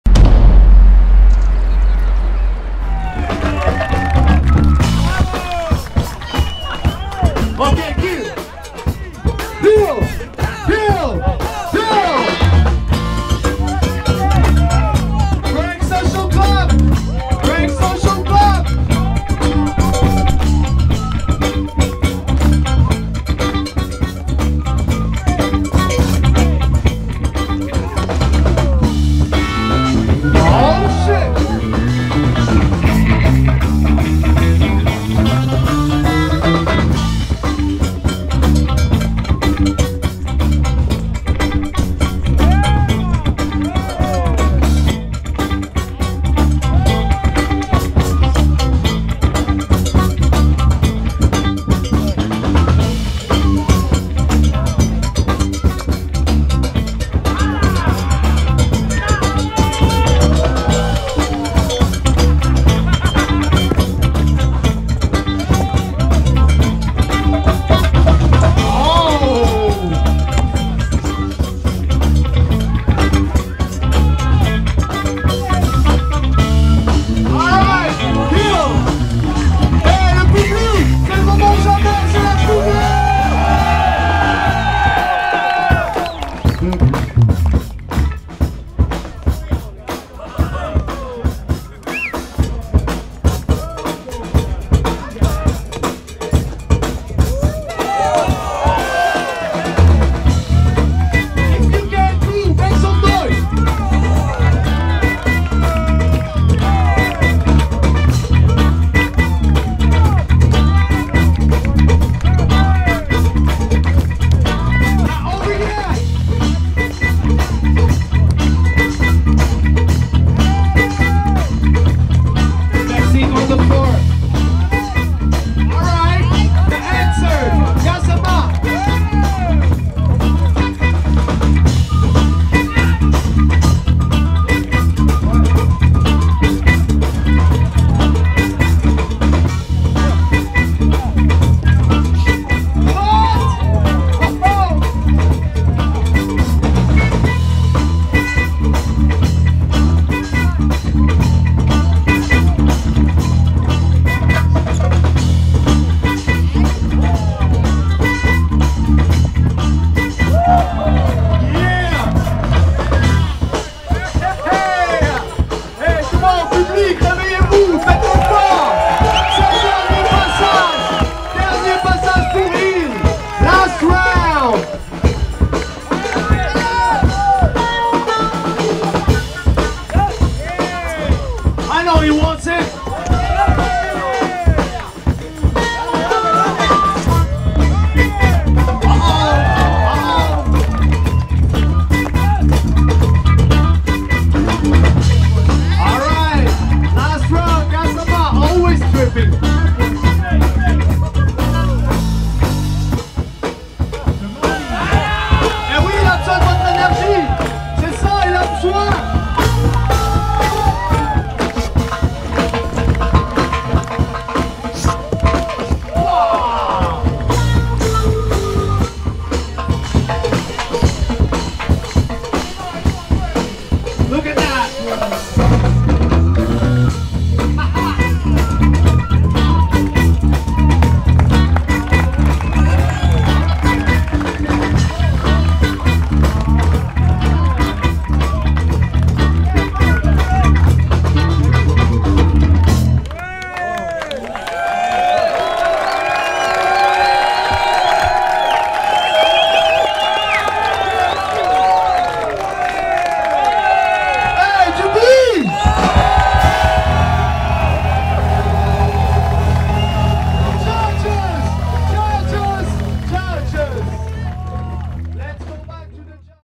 Gassama (Def Dogz) VS Hill (Unik Breakers) Final
Break Social Club
and
Thabo Rivella Games
presents:

Rivella International Battle 1vs1
Gassama (Def Dogz) VS Hill (Unik Breakers)

Winner: Hill